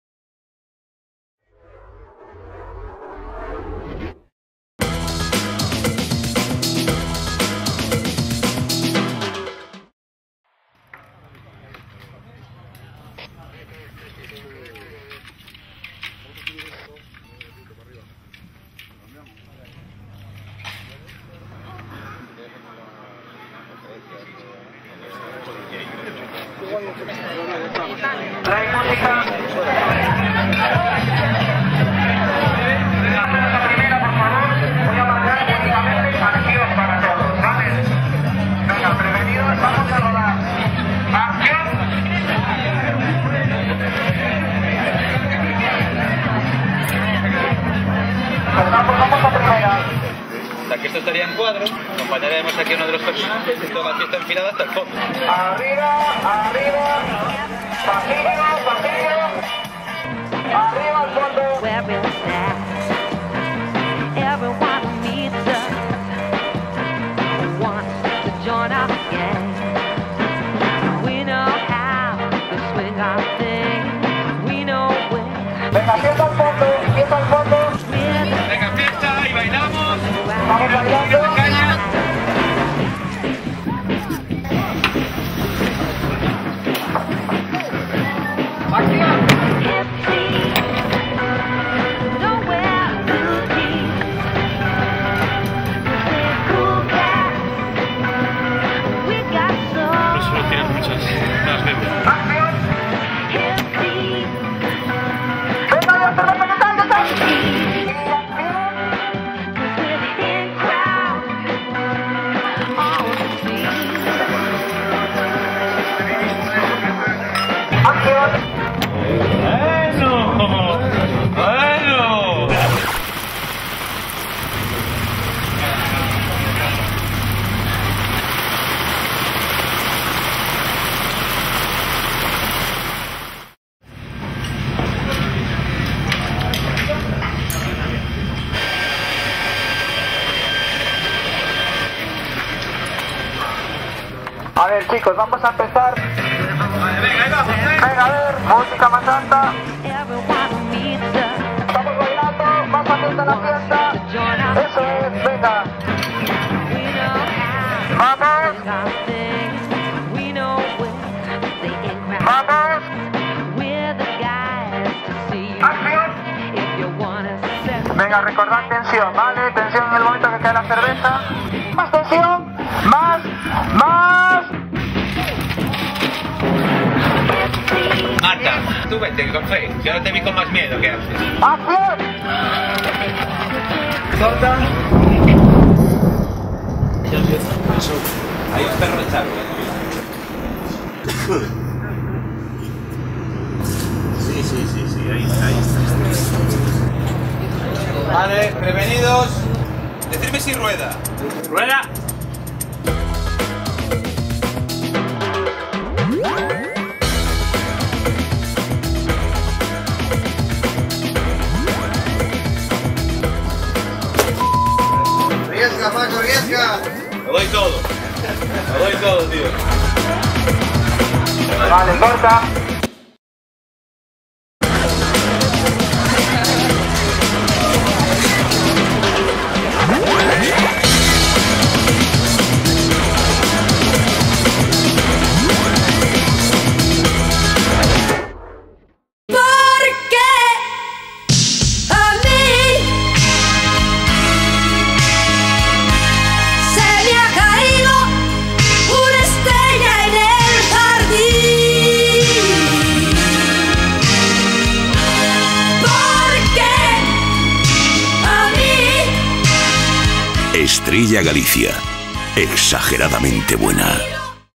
Estrella Galicia, Exageradamente Buena | Making of
Cuando una cerveza es así, no se puede perder ni una sola gota. Una fiesta, amigos, música y Estrella Galicia, pero algo está punto de suceder... Entra y descúbrelo.